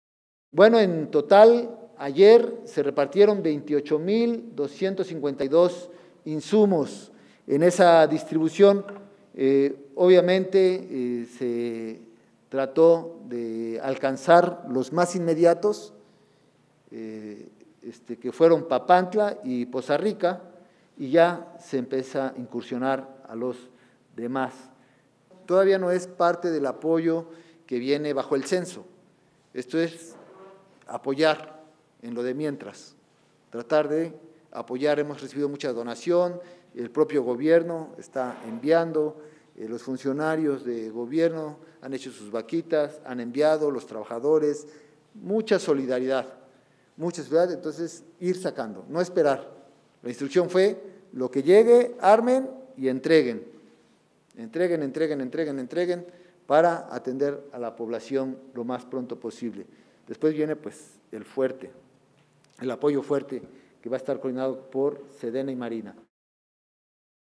Más de 28 mil insumos para habitantes de las regiones Huasteca Baja y Totonaca afectados por Grace
Xalapa, Ver., 27 de agosto de 2021.- En conferencia de prensa, el gobernador Cuitláhuac García Jiménez dio a conocer el avance de los trabajos, acuerdos y asistencia en las regiones Huasteca Baja y Totonaca, a fin de restablecer las comunicaciones y el bienestar de la población más aquejada por el Huracán Grace, que al momento ha recibido más de 28 mil insumos básicos de alimentación.